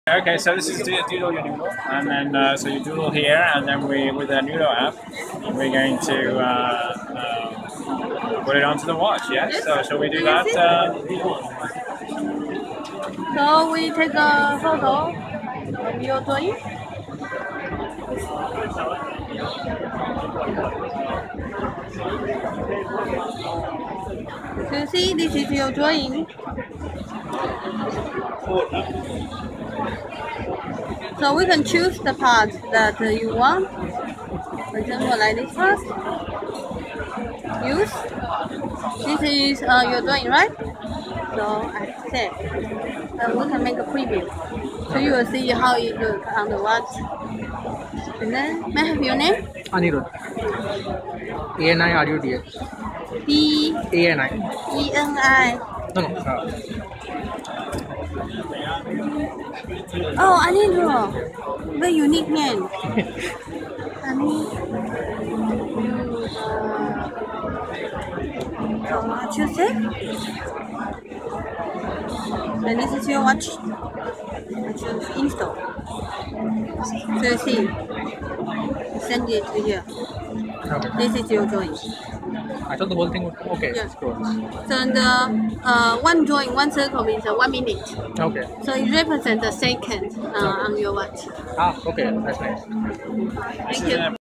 Noodoe Watch - Hands-on
We have a look at Noodoe Watch during Computex 2015, that lets you transfer your drawings via Bluetooth over to the device, to be displayed on the watch face.